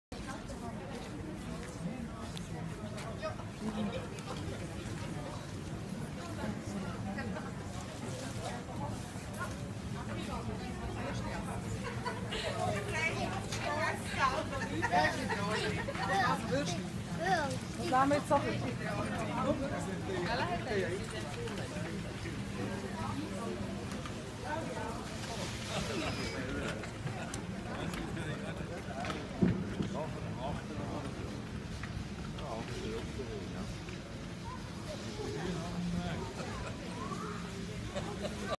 スイス発 ルツェルン市内のクリスマスマーケットで食べ歩き【スイス情報.com】
ルツェルン市内でクリスマスマーケットが開催中。ラクレット、カリーヴルスト、ピザ、フラムクーヘンなどなど、食べ歩きもいいですね。

Y.M.

3歳から高校までのボーディングスクール（寄宿学校）サポート